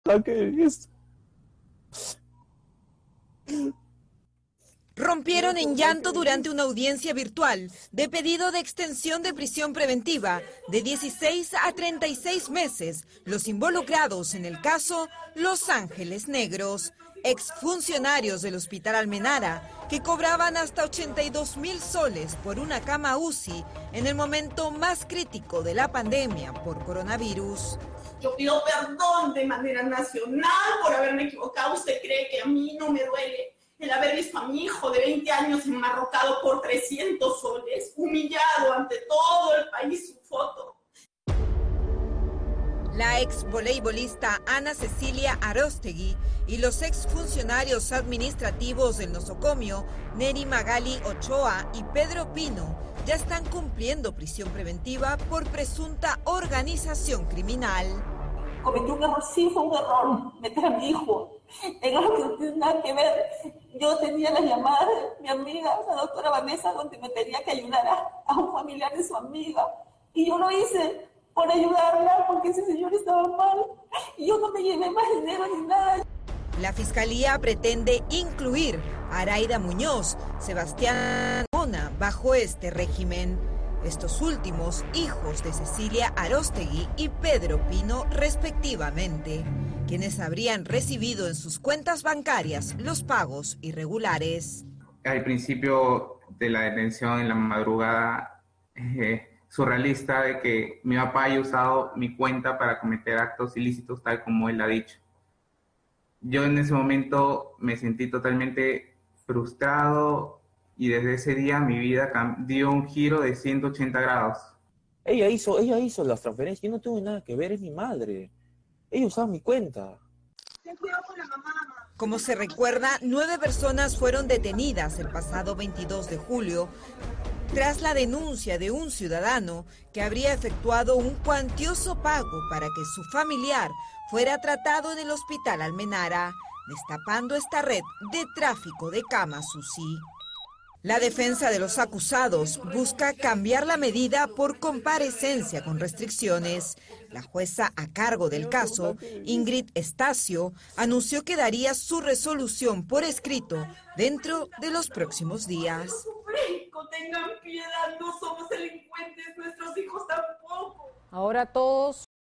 “Los Ángeles Negros del Almenara” lloraron en audiencia de apelación de prisión preventiva
Los involucrados en el caso “Los Ángeles Negros”, ex funcionarios del Hospital Almenara, que cobraban hasta 82 mil soles por una cama UCI en el momento más crítico de la pandemia por coronavirus, rompieron en llanto durante una audiencia virtual de pedido de extensión de prisión preventiva de 16 a 36 meses.

Como se recuerda, 9 personas fueron detenidas el pasado 22 de julio tras la denuncia de un ciudadano que había efectuado un cuantioso pago para que su familiar fuera tratado en el Hospital Almenara, destapando esta red de tráfico de camas UCI.

La exvoleibolista Ana Cecilia Arostegui, y los exfuncionarios administrativos de nosocomio; Nery Magaly Ochoa y Pedro Pino ya están cumpliendo prisión preventiva por presunta organización criminal.

La Fiscalía pretende incluir a Iraida Muñoz, Sebastián Bazalar y Piero Pino Carmona bajo este régimen, estos últimos hijos de Cecilia Arostegui y Pedro Pino respectivamente, quienes habrían recibido en sus cuentas bancarias los pagos irregulares.

La defensa de los acusados busca cambiar la medida por comparecencia con restricciones. La jueza a cargo del caso, Ingrid Estacio anunció que daría su resolución por escrito dentro de los próximos días.